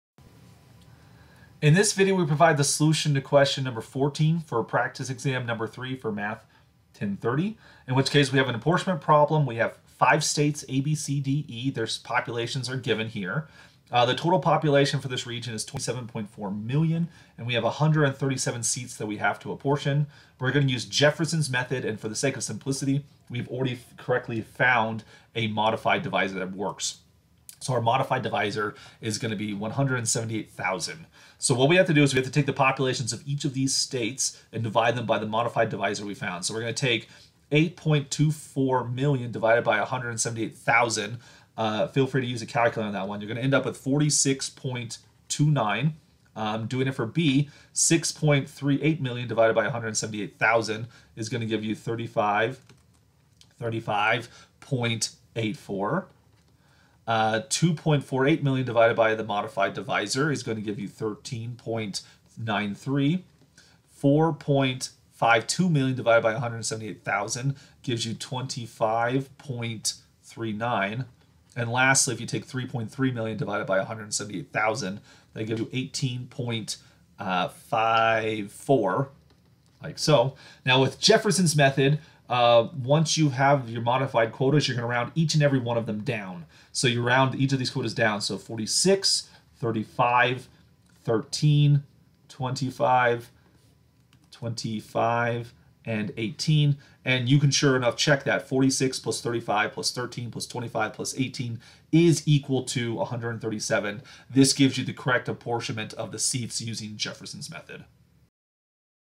Math 1030, Exam 3 - Question 14 (Jefferson's Method)
We apportion a number of seats among states using Jefferson's method.